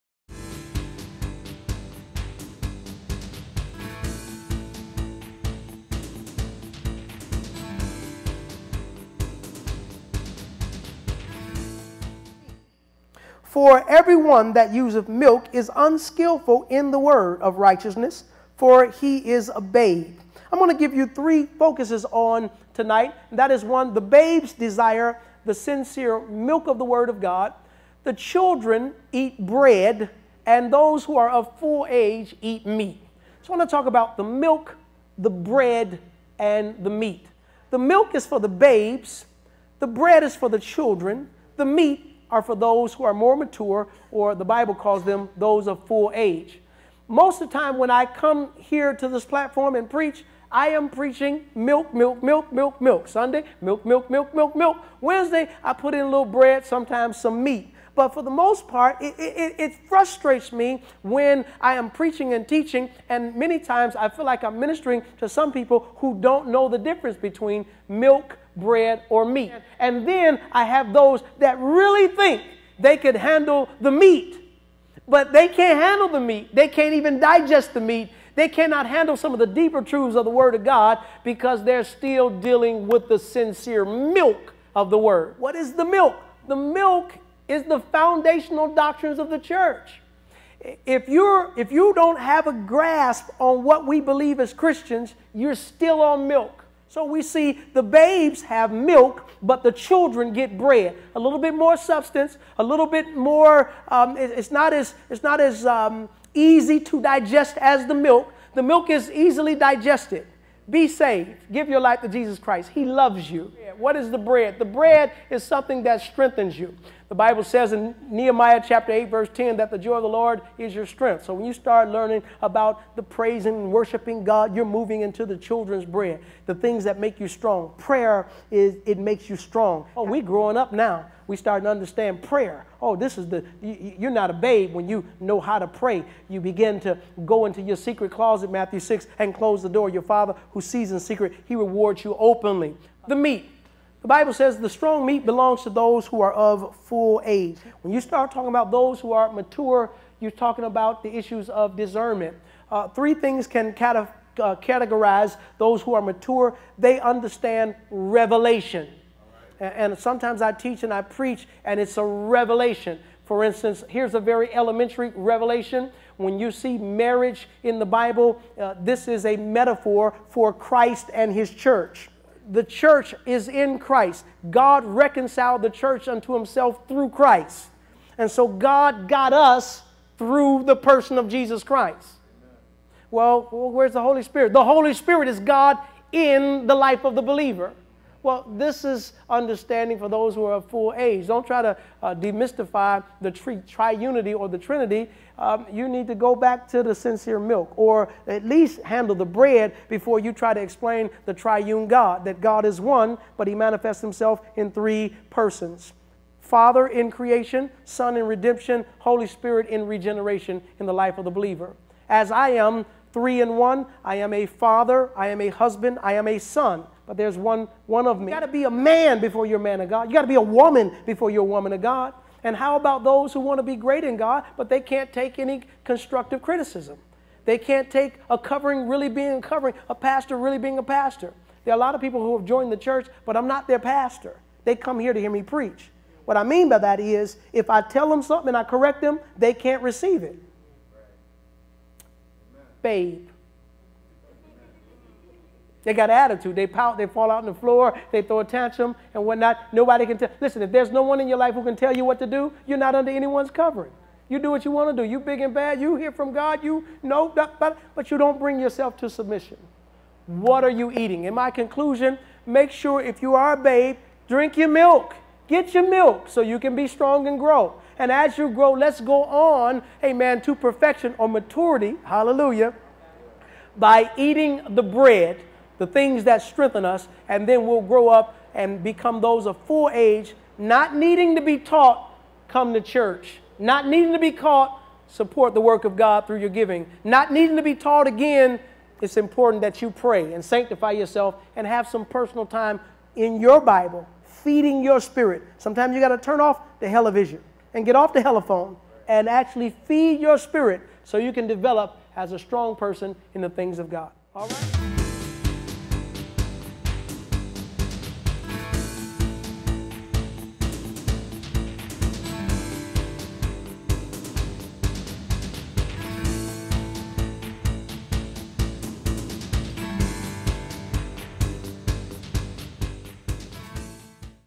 Bishop Eric K. Clark - "What Have You Been Eating?"
Bishop Eric K. Clark preaches on What have you been eating?